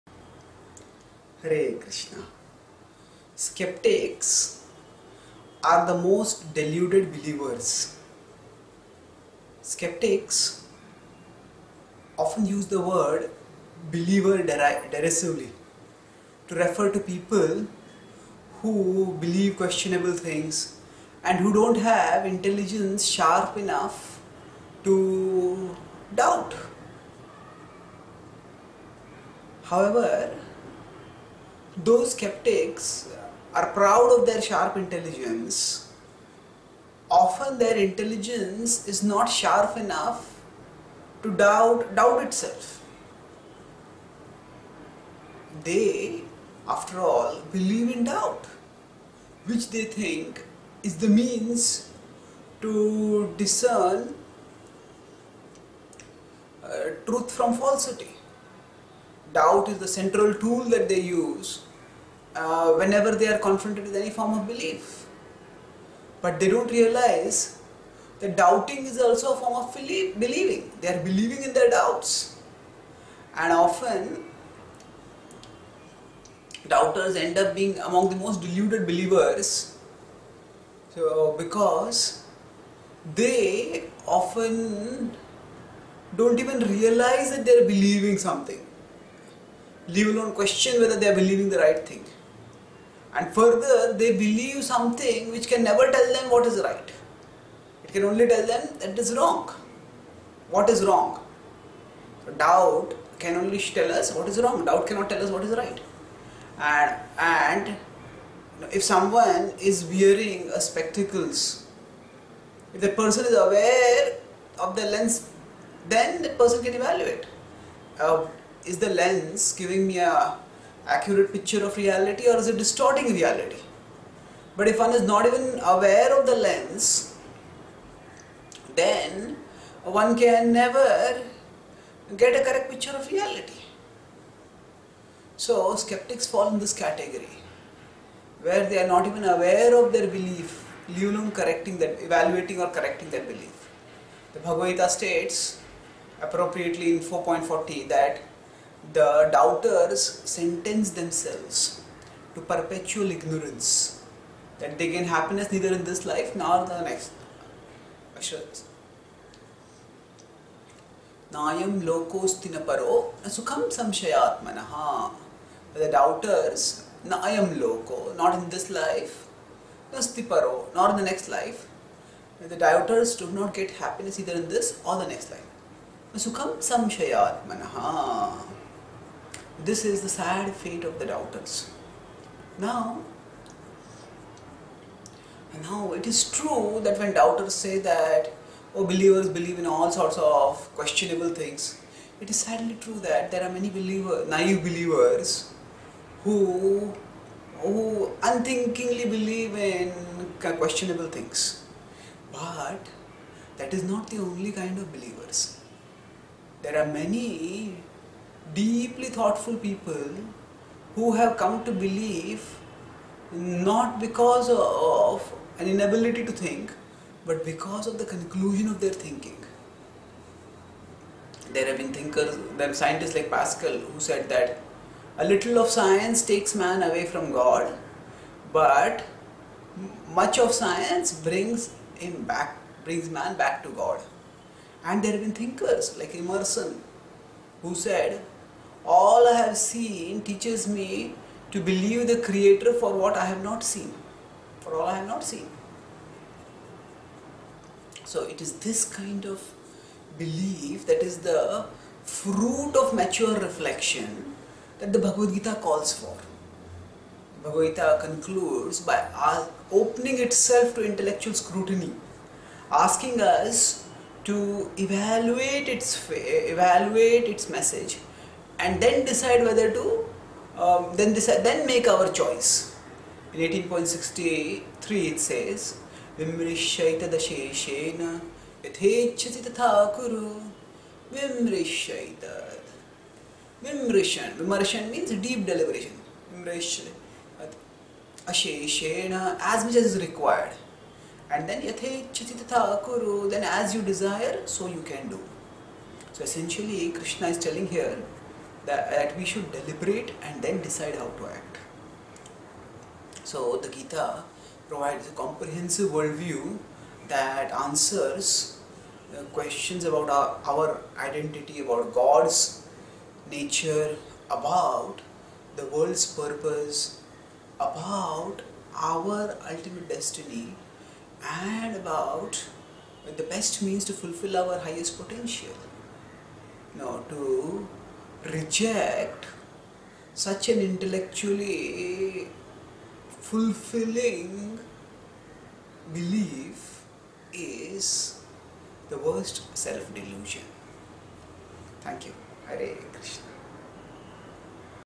Doubters are the most deluded believers
Based on Bhagavad Gita Chapter 18, Text 63, By Chaitanya Charan Das
Article - Skeptics often deride believers for not being sharp enough to doubt questionable ideas.

Though skeptics may have sharp intelligence that is well used to doubt fallacious beliefs, their intelligence is often not sharp enough to doubt doubt itself, which is the foundation of their skepticism. They overlook the fact that being skeptical essentially means believing one’s doubts. So, such doubters often risk becoming deluded believers of doubt.
Deluded firstly because they believe in something that keeps them forever in ignorance – doubt can only tell what is wrong, never what is right. And secondly because they often don’t recognize that they are believers too. People who are aware that they are wearing a lens can check its color and the accuracy of what they see through it, those who aren’t aware of their lens can’t check their vision. Such is often the skeptics’ plight.

Still, skeptics are right in criticizing the unthinking believing that characterizes many believers. They may be surprised, however, to know that not all believers are unthinking – many thoughtful people have chosen belief after intelligent contemplation, as, for example, scientists like Pascal (“Little science takes you away from God, but more of it takes you to him”) and thinkers like Emerson (“All I have seen teaches me to trust the Creator for all I have not seen”).

The Gita recommends this kind of deeply considered belief when it urges us (18.63) to deliberate its message before making our choice. Gita wisdom offers a comprehensive worldview that explains our identity, God’s nature, the world’s purpose, our ultimate destiny and the best means to achieve our highest potential. And it also delineates a time-honored yogic process for gaining experiential realization of higher spiritual realities.

Why should anyone let a miscalculated belief in doubt deprive oneself of such an intellectually -stimulating expansion of consciousness?